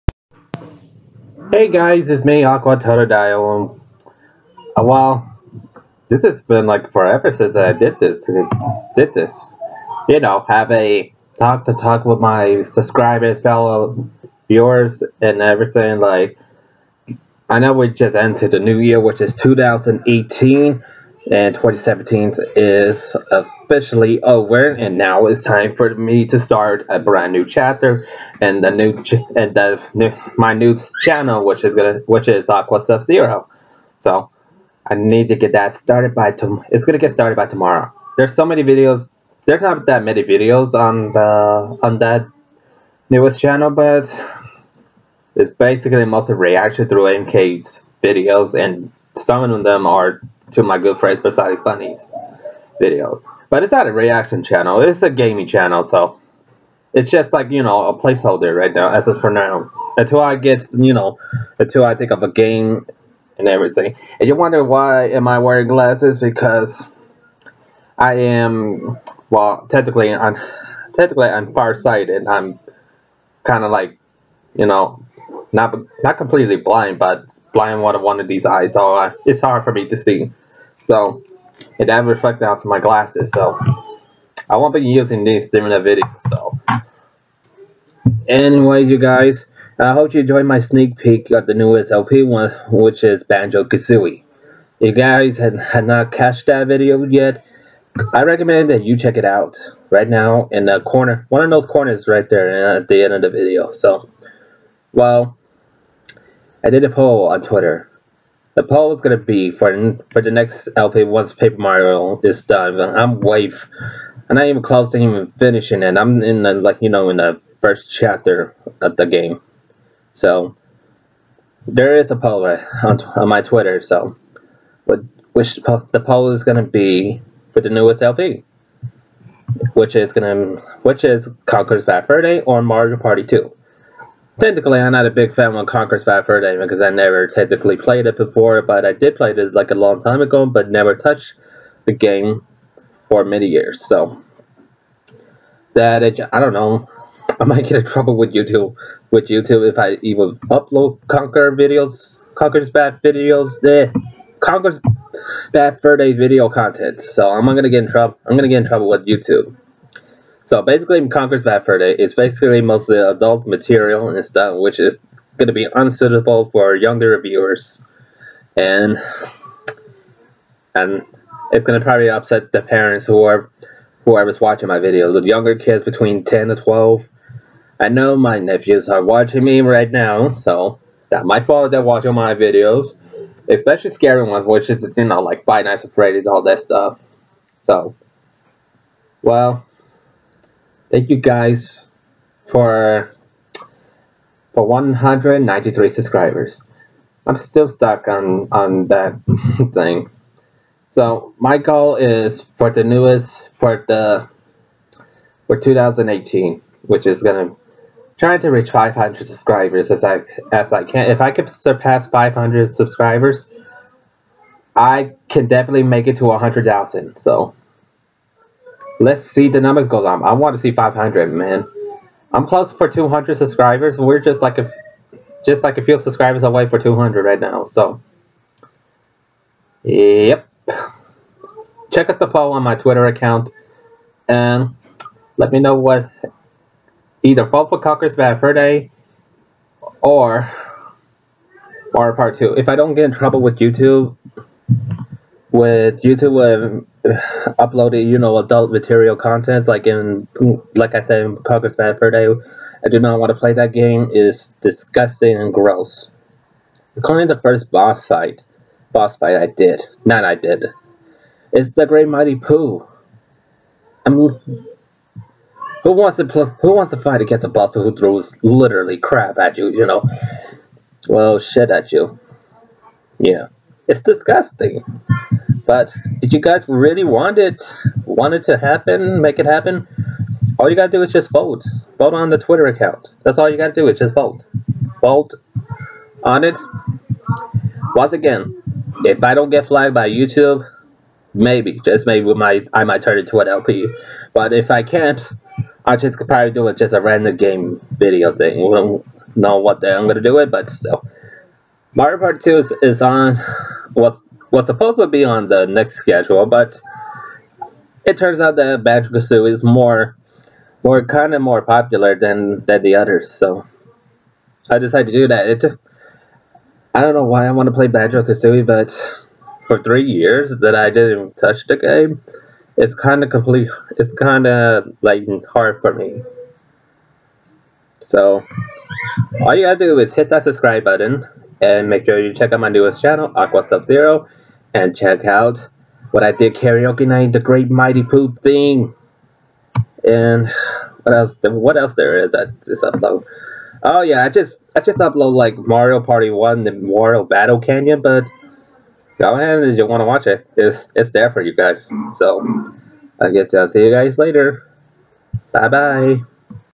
The New Year 2018
Hey guys welcome it a brand new year for me and for my channel's there gong to new project videos on my new Aqua Sub-zero and same goes for the rest of my channel's hope you enjoy the brand new year oh one more thing thanks for 193 subscribers let's keep the numbers going up until i see 500 subscriber's